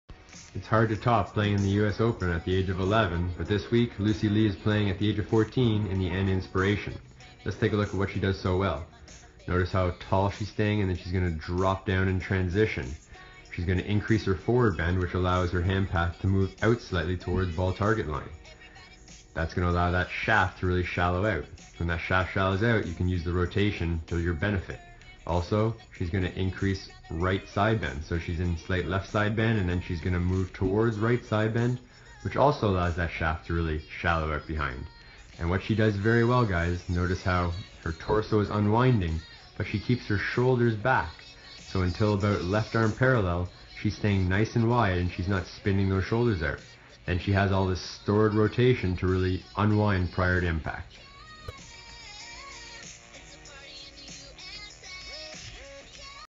How to shallow out like Lucy Li in golf
Luci Li showing great promise through fantastic sequencing avoiding an early upper body spinout. She increases ground pressure in transition loading into the ground increasing hip flexion. She shallows out increasing her forward bend, and moving toward right side bend. Shallow the shaft out if you want to use the benefits of the stored upper body rotation.